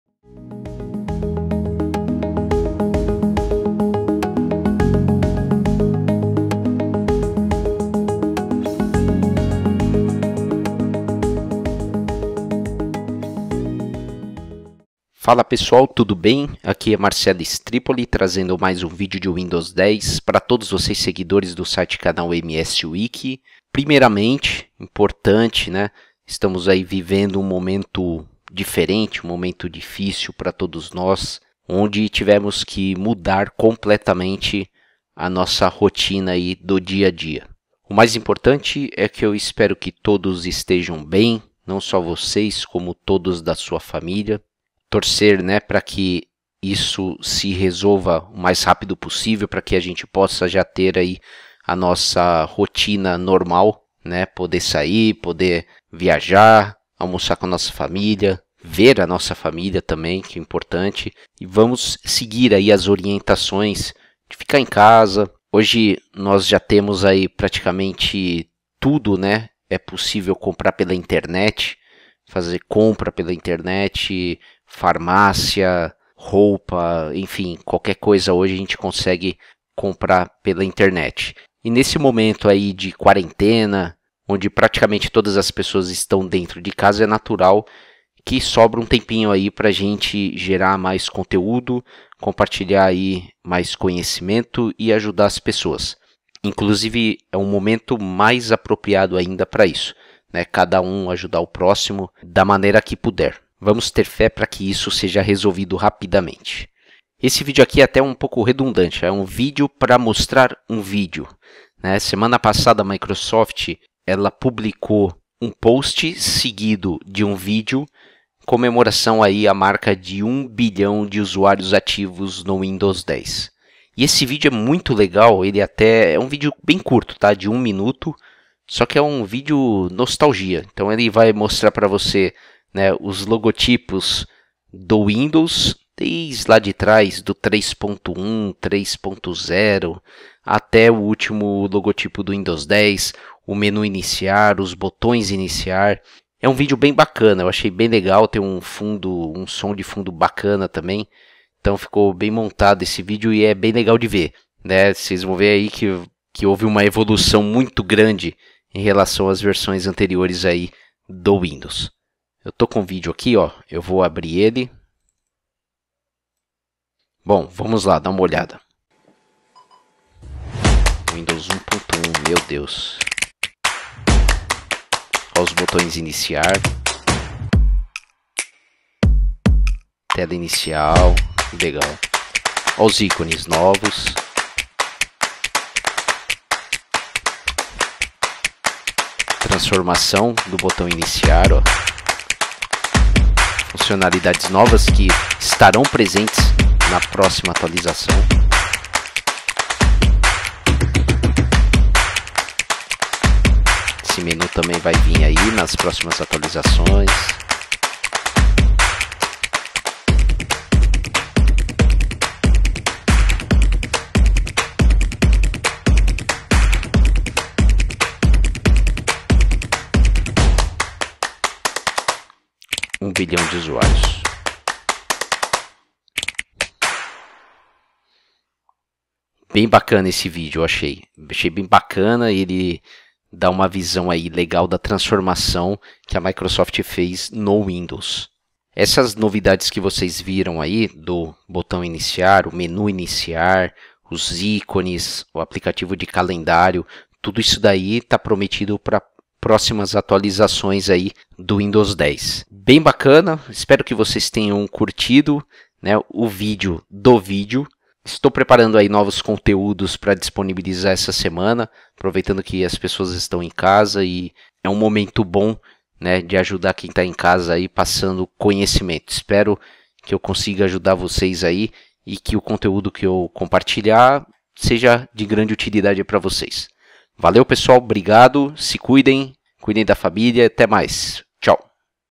Windows 10 - A evolução do Windows. Novo botão iniciar, novos ícones, novo tema e muito mais.
Windows 1.0, 3.0, 95, 2000, 98, XP, Vista, 7, 8, 8.1 e Windows 10. Você se lembra dessa evolução? Como eram os botões iniciar? Menu iniciar? Os logotipos?
Semana passada a Microsoft publicou um artigo seguido de um vídeo comemorando 1 bilhão de usuários ativos. O vídeo de 1 minuto mostra a evolução das versões do Windows e aproveita para mostrar as novidades que virão nas próximas atualizações do Windows 10, como o novo menu iniciar, novos ícones, tema, calendário e muito mais. Vídeo bem bacana.

Fiquem com Deus, cuidem-se e cuidem da familía também.